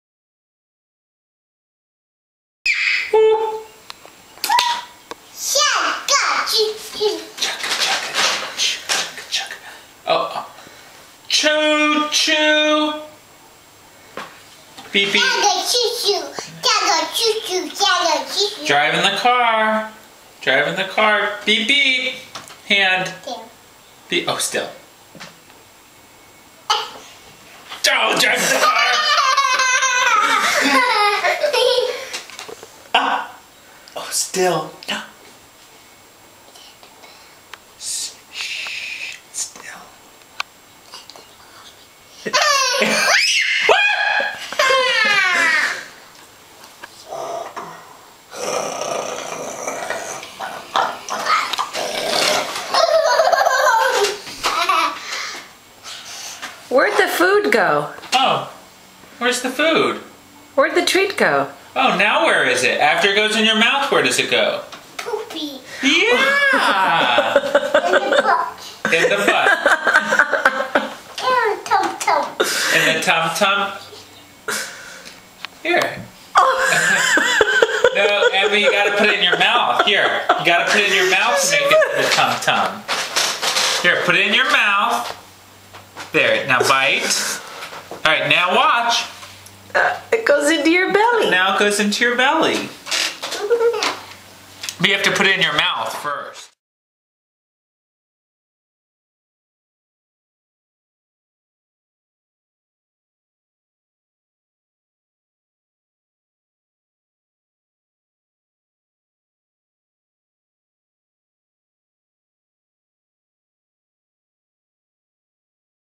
Toddler Digestion 101
A typical playtime and dinner conversation between Daddy and Emily